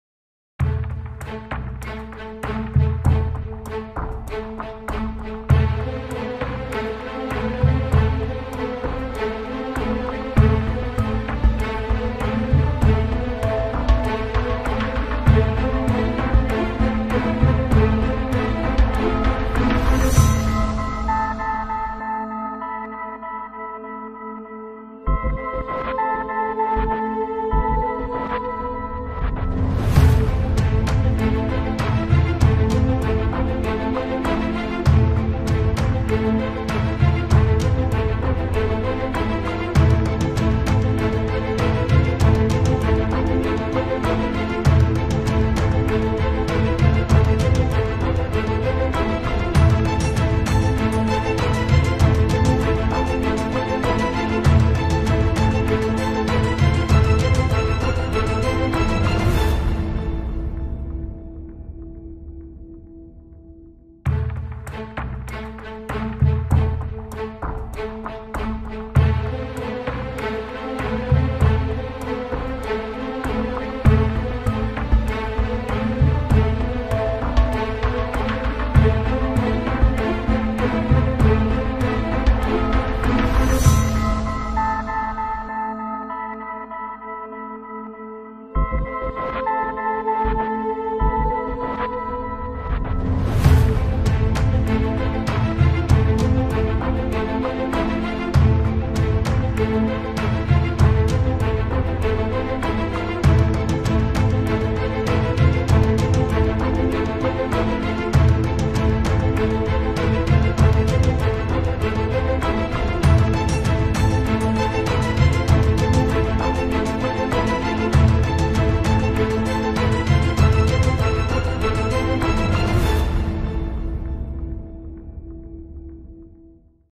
Warrior Breakdowns
Hi this is not detailed breakdowns just enjoy some glimpse of unreal engine.